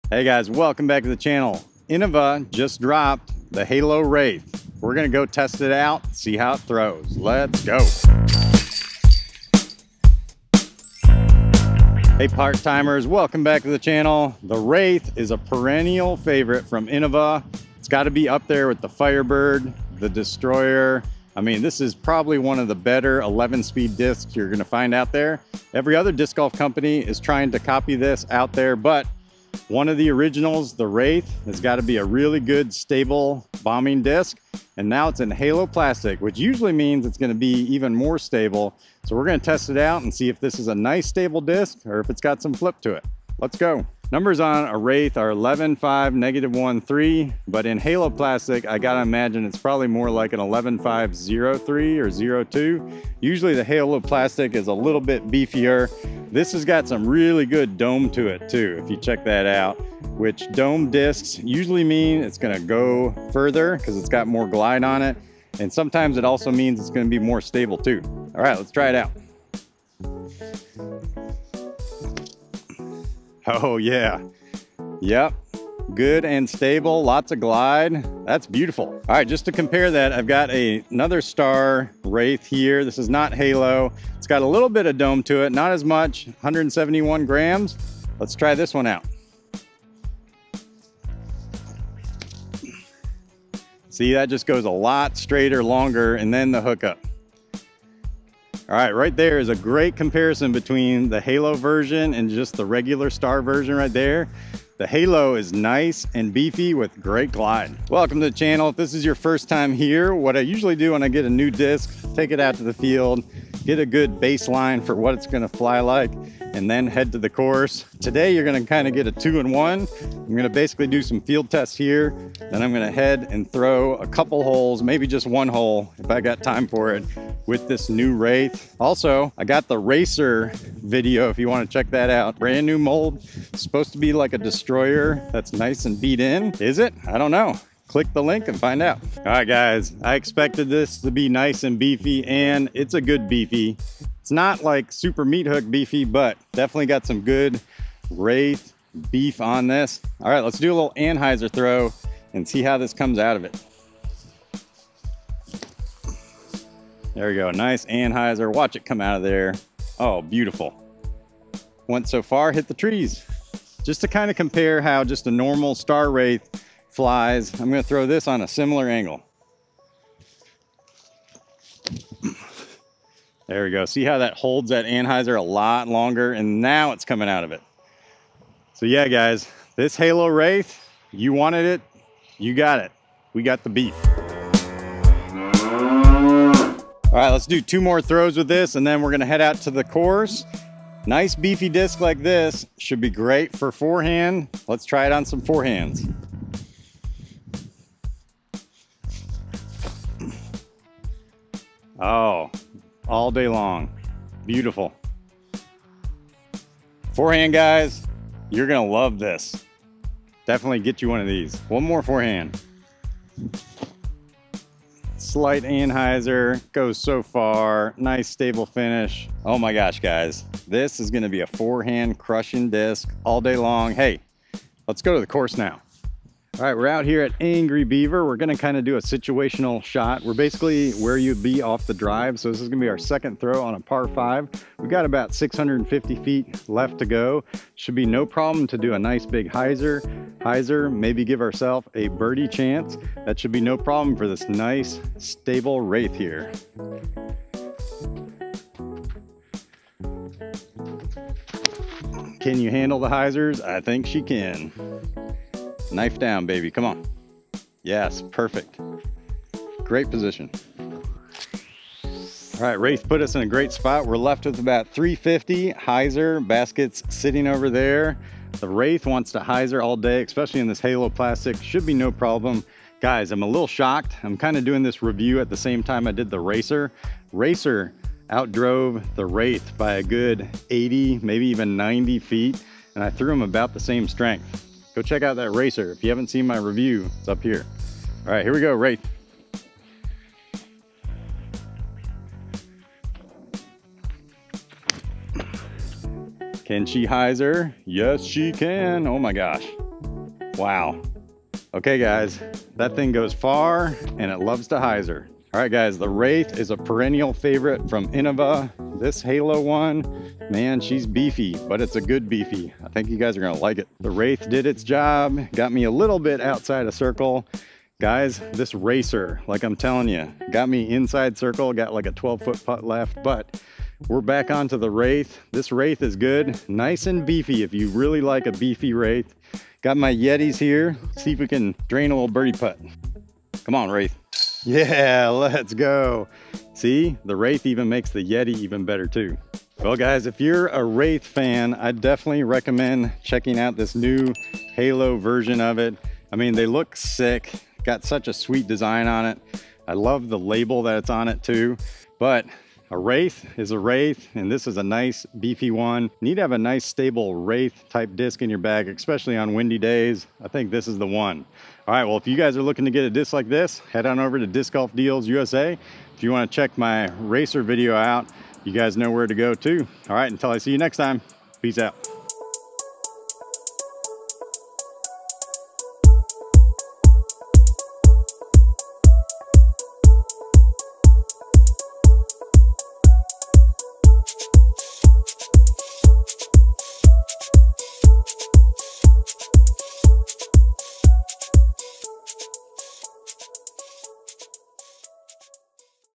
NEW HALO WRAITH // Innova Disc Golf Review
The HALO WRAITH just dropped from Innova Disc Golf.  The flight numbers are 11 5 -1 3.  How does it fly?  Let's find out!

MY CAMERA GEAR:
-Lav Microphone
-Shotgun Microphone